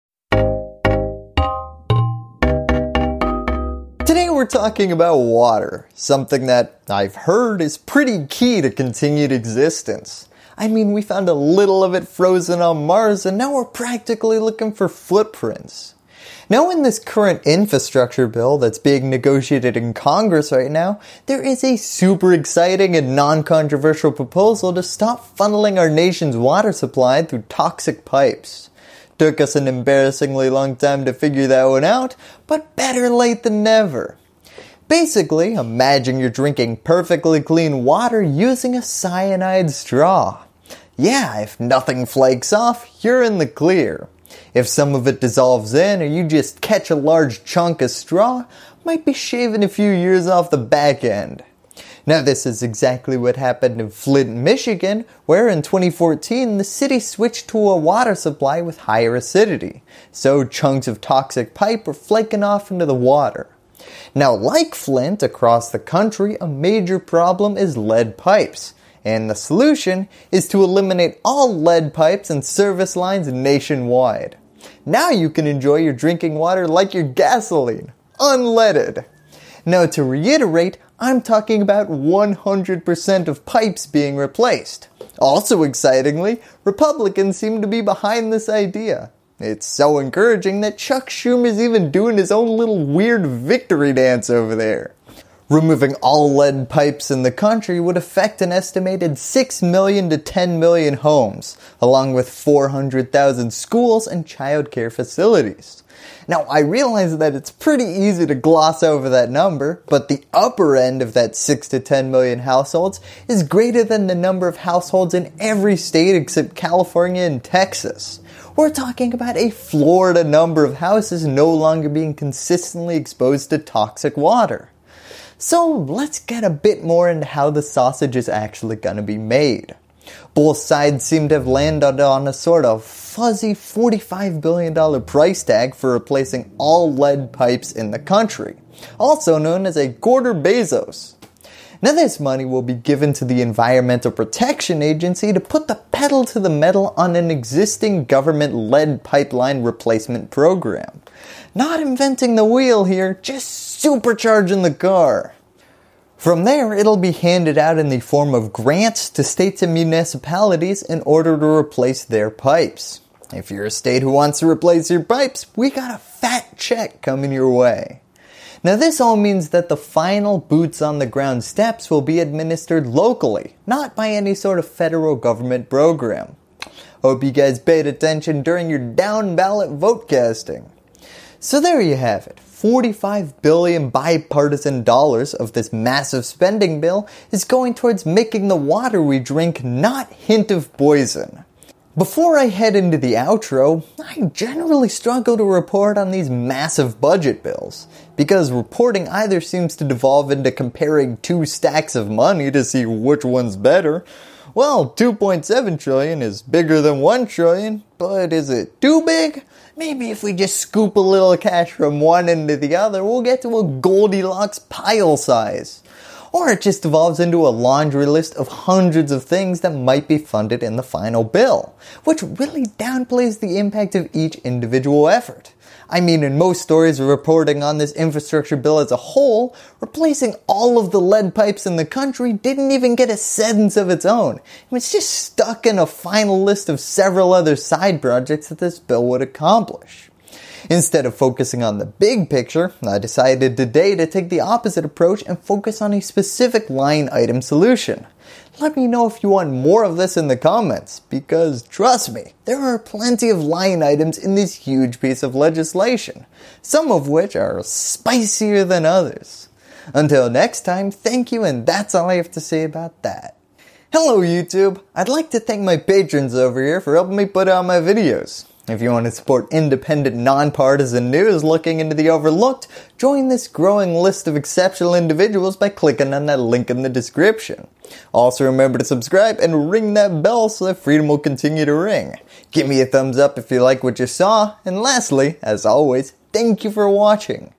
The Plan to Replace All Lead Pipes in America, Explained
From Flint, Michigan to many other small towns across the country we haven’t heard about, lead pipes have had a negative impact on millions of households. The infrastructure plan making its way through congress contains $45 billion dollars to replace all lead pipes in America. Here is exactly what’s happening.